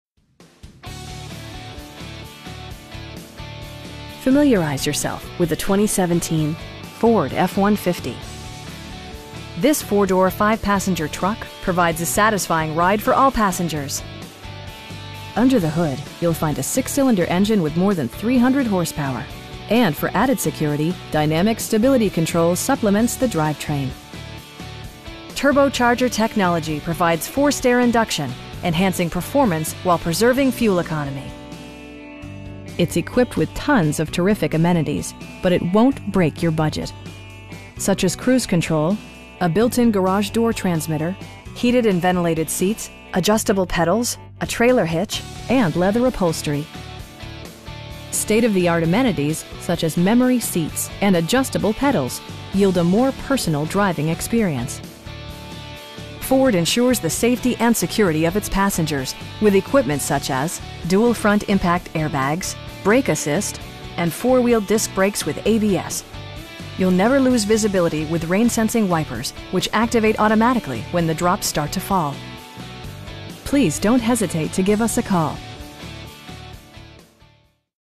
2017 Ford F-150 Lariat in Sioux Falls, SD 57106
4901 W. 26th St

Familiarize yourself with the 2017 Ford F-150. This 4 door, 5 passenger truck provides a satisfying ride for all passengers. Under the hood you'll find a 6 cylinder engine with more than 300 horsepower, and for added security, dynamic Stability Control supplements the drivetrain. Four wheel drive allows you to go places you've only imagined. Turbocharger technology provides forced air induction, enhancing performance while preserving fuel economy. It's equipped with tons of terrific amenities, but it won't break your budget. Such as cruise control, a built-in garage door transmitter, a power seat, heated and ventilated seats, telescoping steering wheel, adjustable pedals, a trailer hitch, and leather upholstery. For drivers who enjoy the natural environment, a power moon roof allows an infusion of fresh air. State-of-the-art amenities such as memory seats and adjustable pedals yield a more personal driving experience. Premium sound drives 10 speakers, providing you and your passengers a sensational audio experience. Ford ensures the safety and security of its passengers with equipment such as: dual front impact airbags, front side impact airbags, traction control, brake assist, a security system, and 4 wheel disc brakes with ABS. You'll never lose visibility with rain sensing wipers, which activate automatically when the drops start to fall. We have the vehicle you've been searching for at a price you can afford. Please don't hesitate to give us a call.